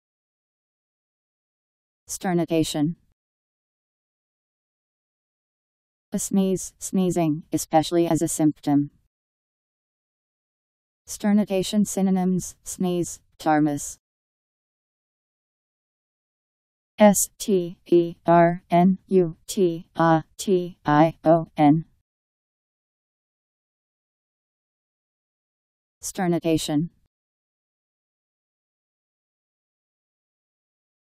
Sternutation Meaning
Video shows what sternutation means. A sneeze; sneezing, especially as a symptom.. sternutation synonyms: sneeze, ptarmus.  Sternutation Meaning. How to pronounce, definition audio dictionary. How to say sternutation. Powered by MaryTTS, Wiktionary